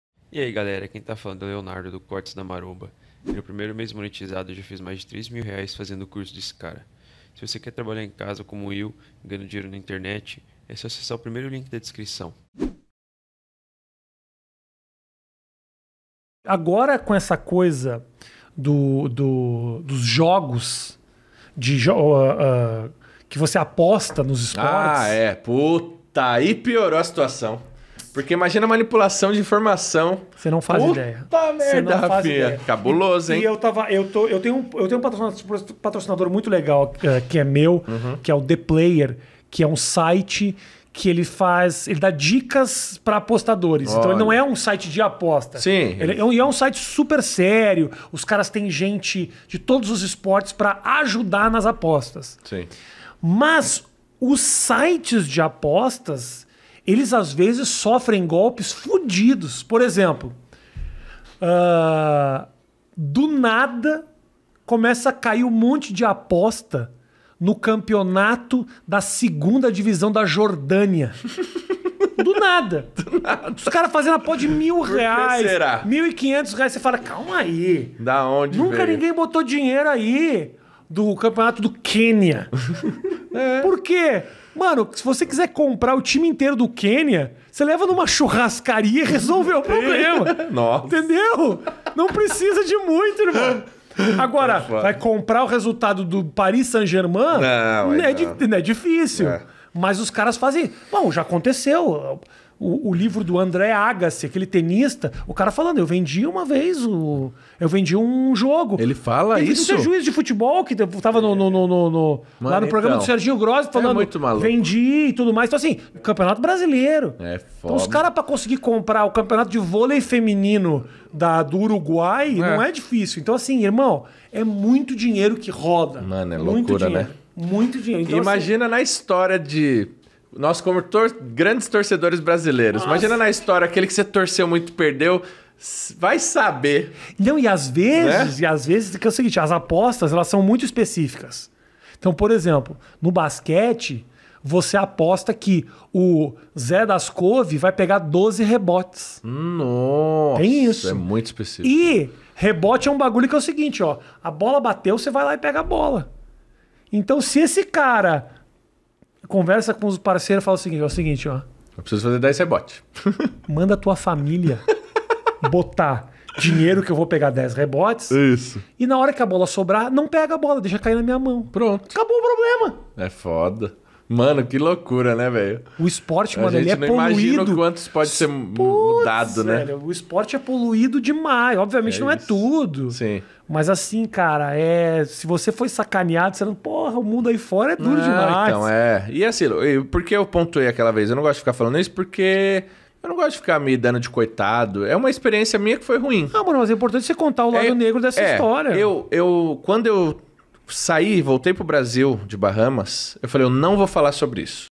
RAFINHA BASTOS ACUSA SITES DE AP*STAS DE SEREM FACILMENTE MANIPUL*D0 😱😱😱 - Cortes da Maromba
Rafinha e Renan discutem sobre sites de ap*sta e resultados.